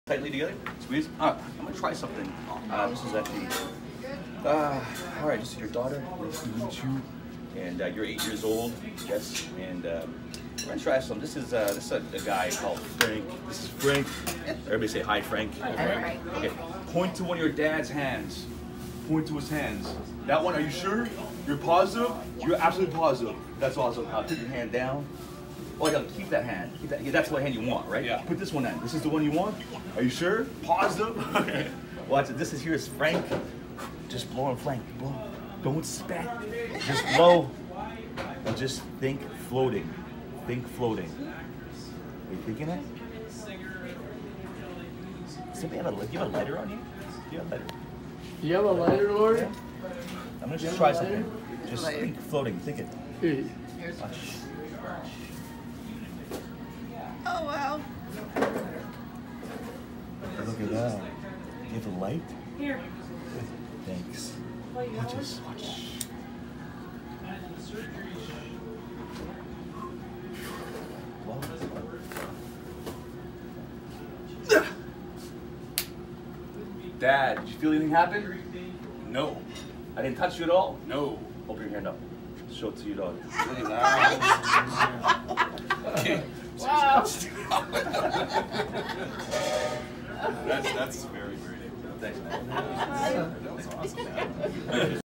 Floating Ashes
Sean Watson Magician performing at Hambos Restaurant in Winnipeg Manitoba 🇨🇦